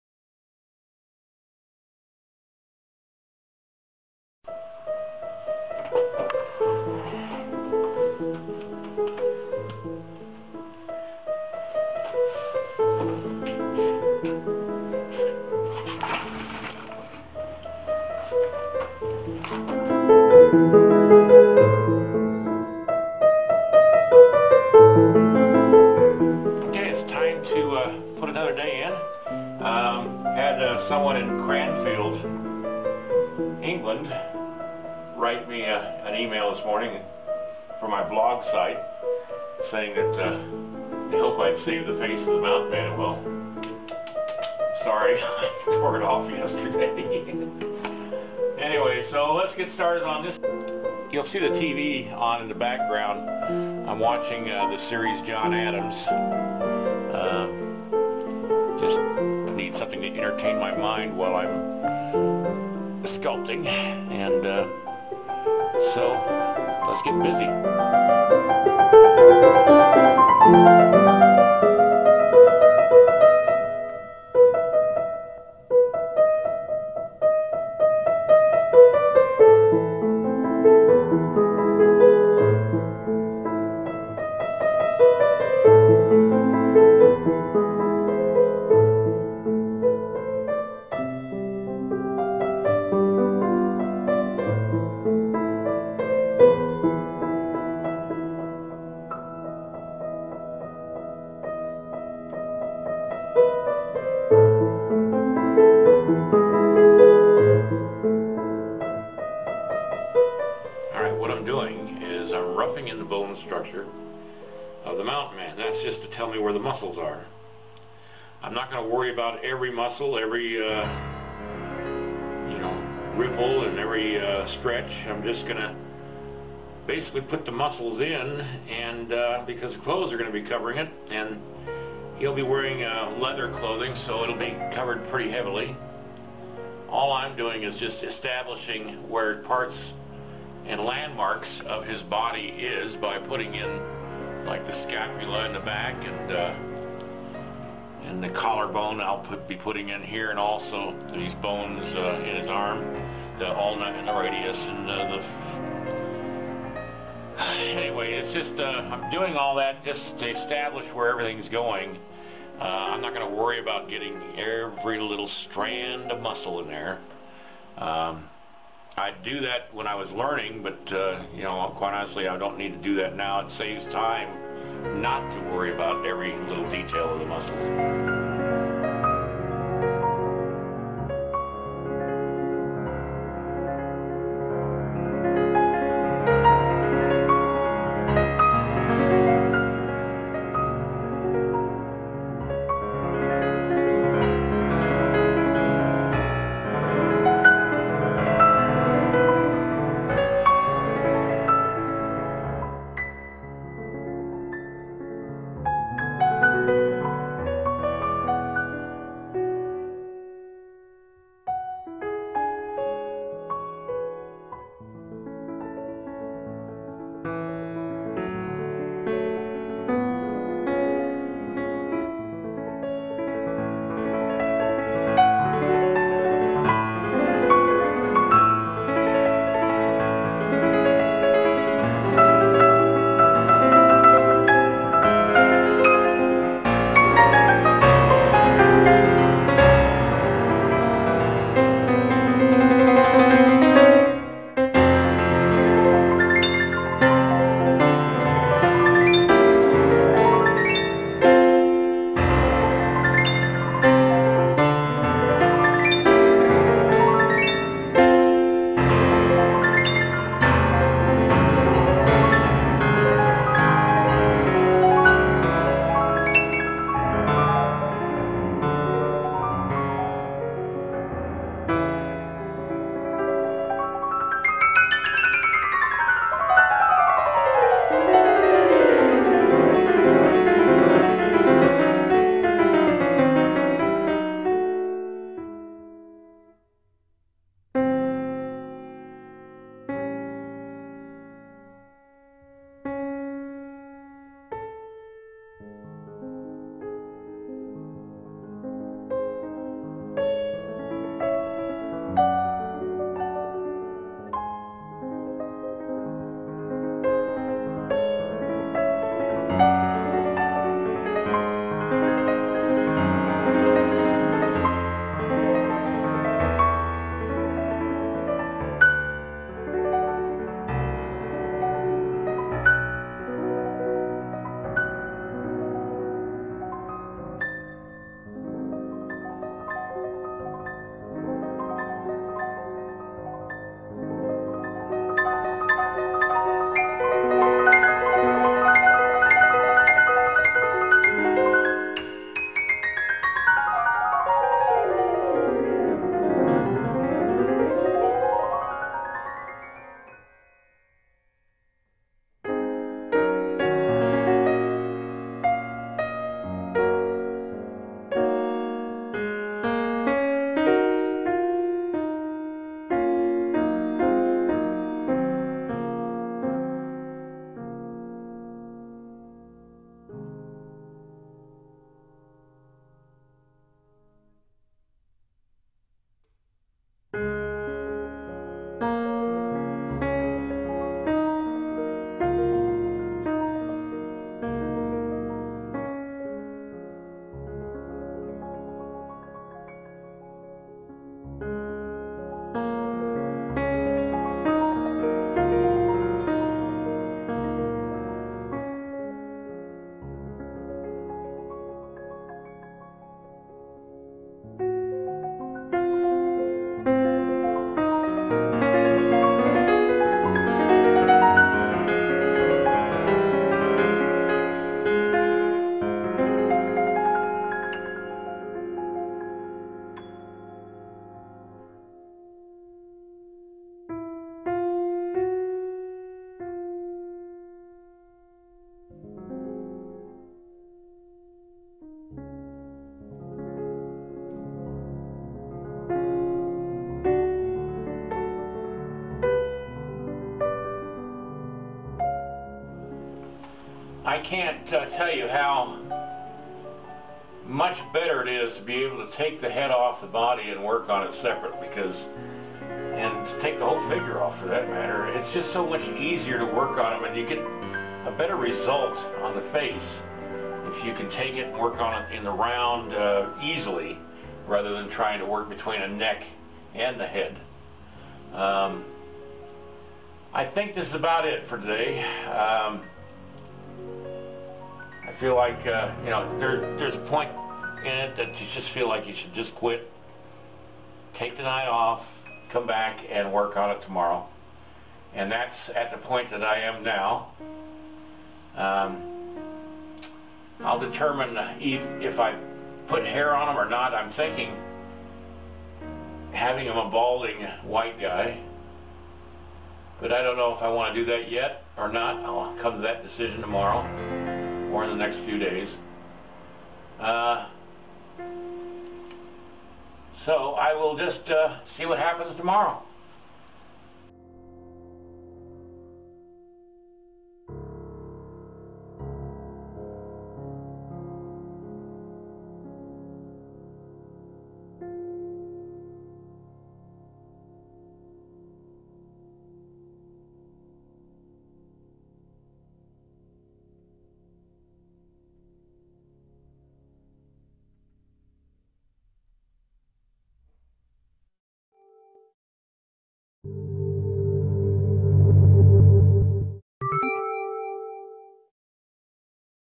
Re-Doing a Face 11/19/09
Today, I worked on part of the upper body of the mountainman and then on his face.